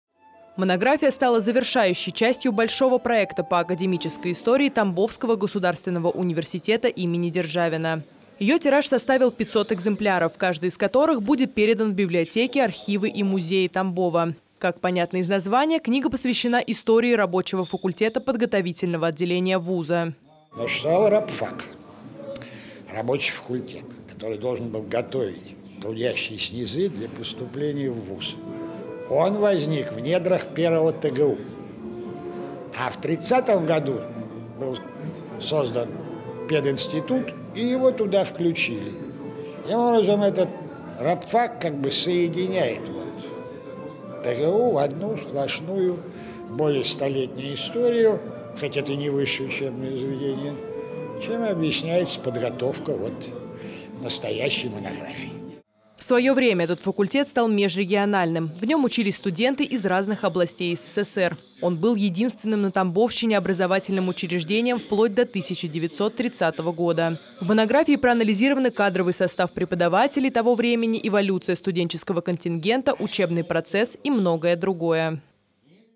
В ТГУ им. Г.Р. Державина презентовали монографию «Тамбовский рабочий факультет. Очерк истории»
Монографию «Тамбовский рабочий факультет. Очерк истории», автором которой стал Александр Аврех, презентовали в Державинском университете. В монографии проанализированы кадровый состав преподавателей, эволюция студенческого контингента, учебный процесс и многое другое.